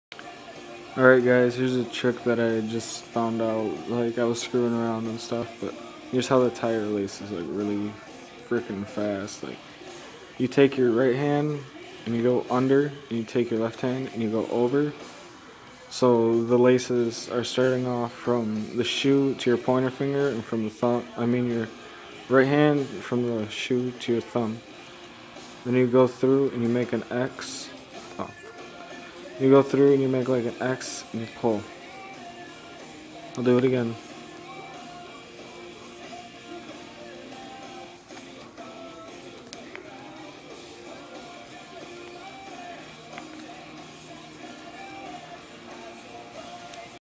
Tie you're shoes in under one second.
I was tieing them wrong all along XD.